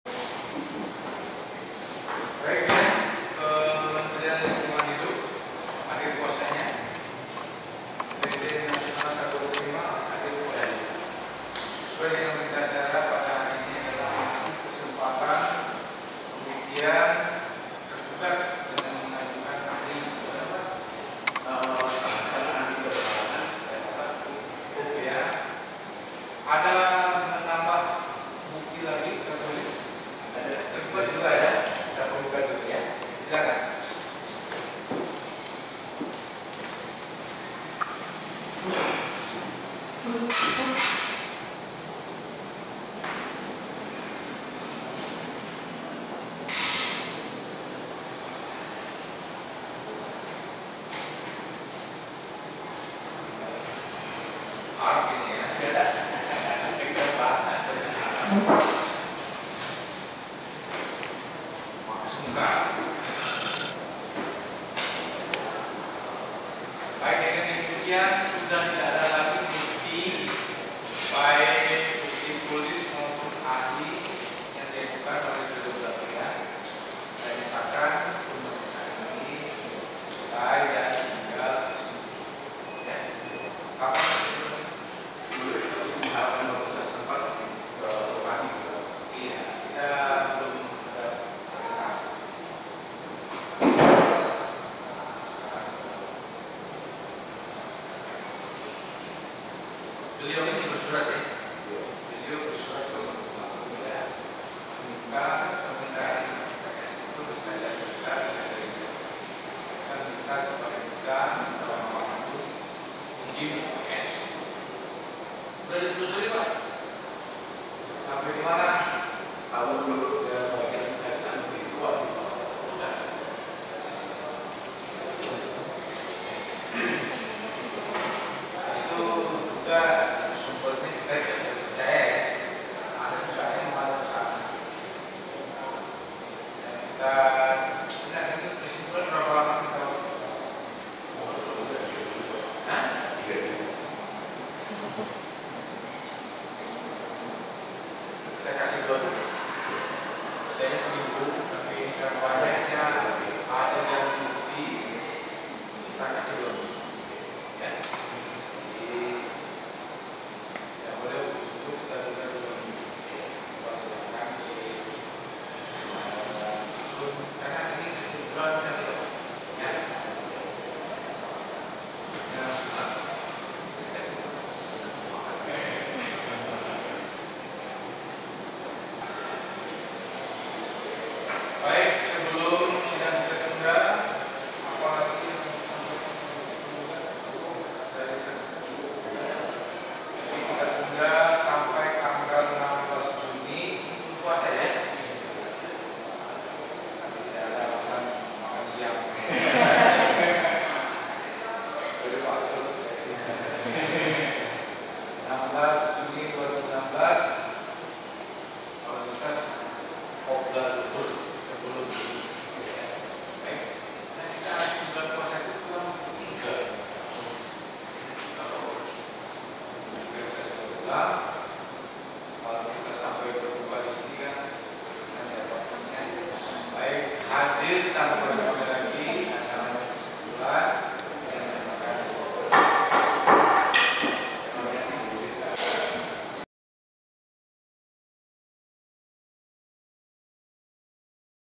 Pembuktian Usai, Sidang Dilanjutkan Ke Agenda Kesimpulan
Pemeriksaan saksi ahli yang mestinya dilanjutkan minggu ini batal terlaksana karena tidak jadi dihadirkan oleh pihak tergugat. Karena itu, minggu ini agenda sidang hanya melengkapi barang bukti dari pihak penggugat dan tergugat. “Hari ini kesempatan terakhir ya. Kalau tidak ada lagi, kita lanjut ke kesimpulan,” kata hakim ketua Muchtar Effendi.